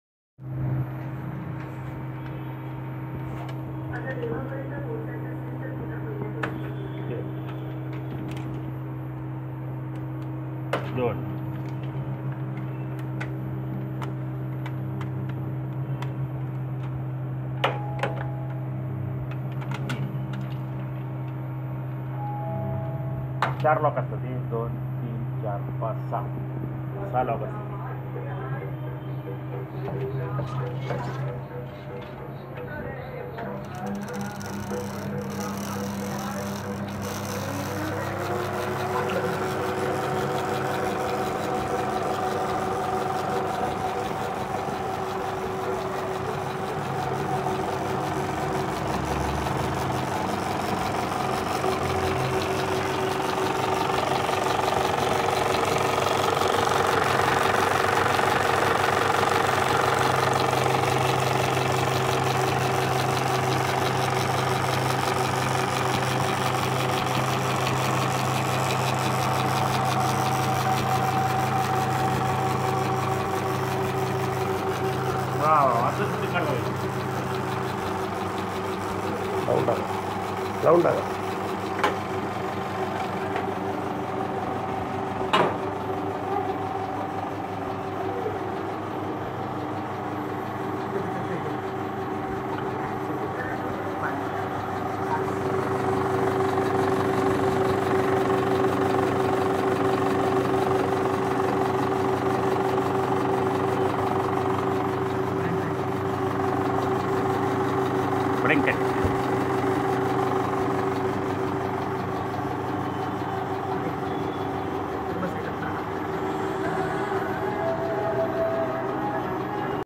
MUST WATCH - Very Rare Encient Film projector..
We used to see movies on such kind of projectors..